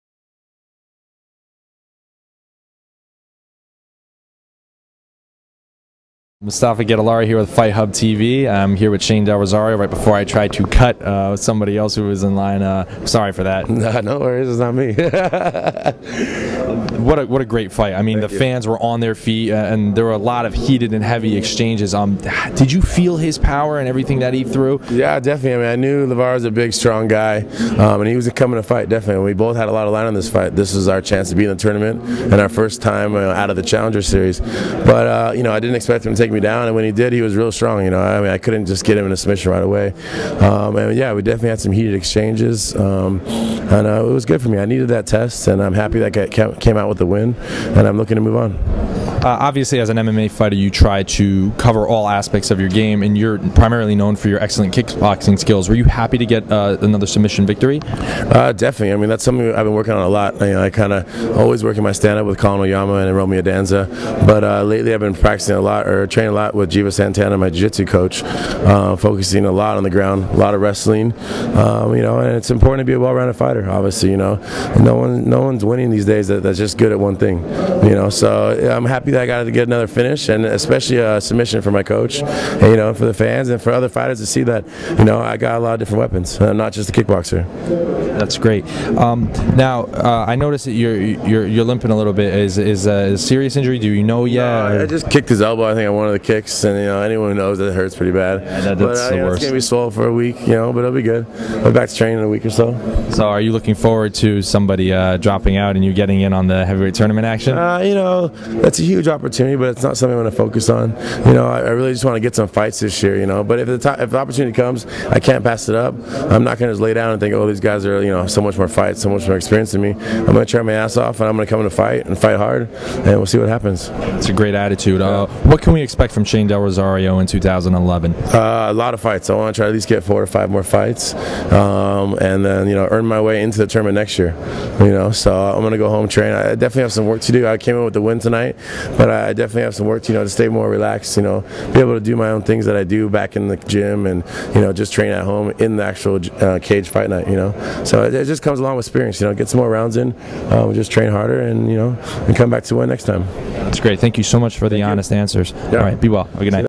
Shane Del Rosario " I am More than Just A Kickboxer Now"
Fight Hub TV caught up with rising heavyweight Shane Del Rosario as he won via armbar against rugged fighter Lavar Johnson.